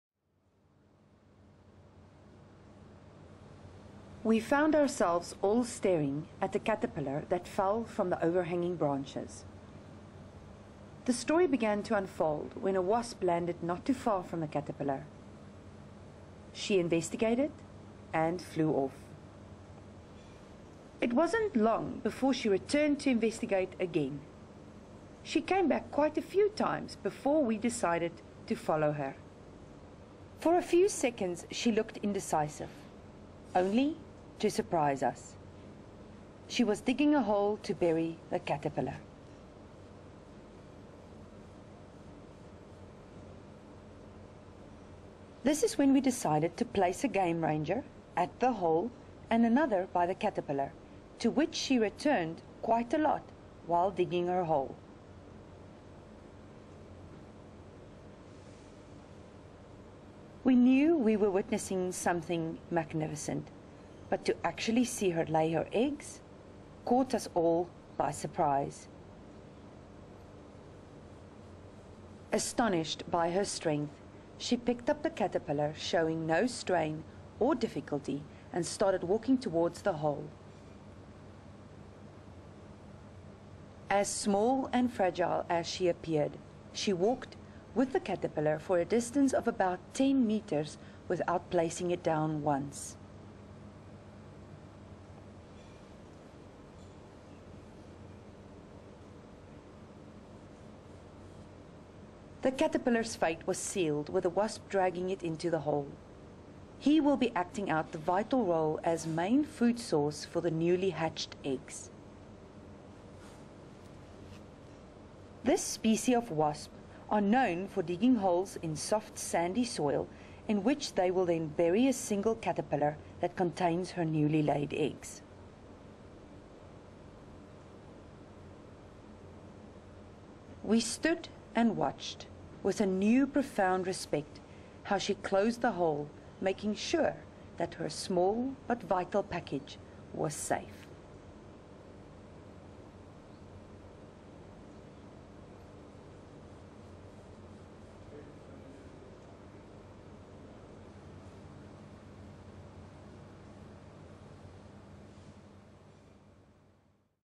Wasp Laying Eggs in a Caterpillar
Watching a wasp lay its eggs in a caterpillar at Sabi Sabi